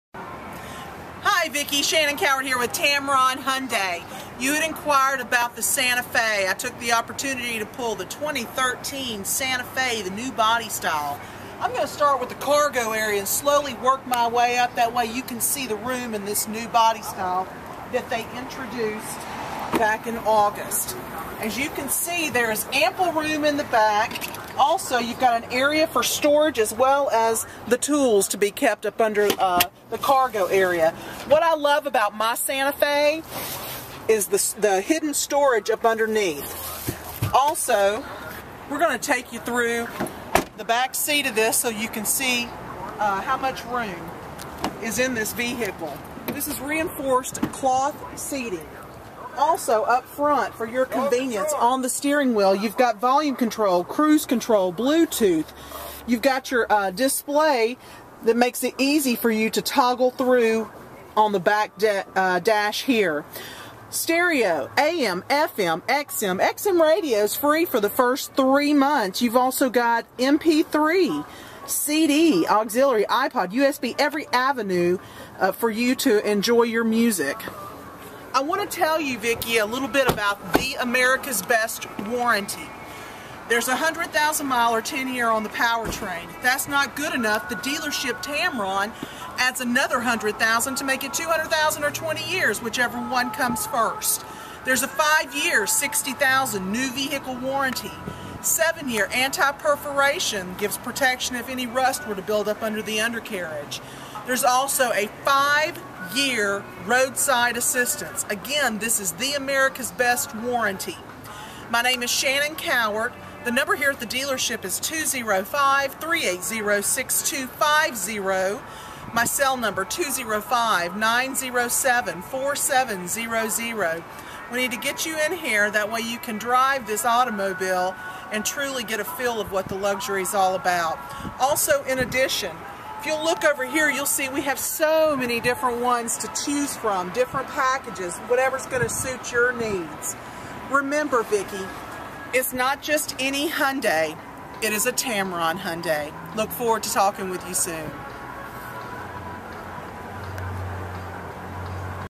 A personal message from Shannon Cowart at Tameron Hyundai
At your convenience, give me a call. I look forward to speaking to you soon. Have a good day.

Shannon Cowart
Tameron Hyundai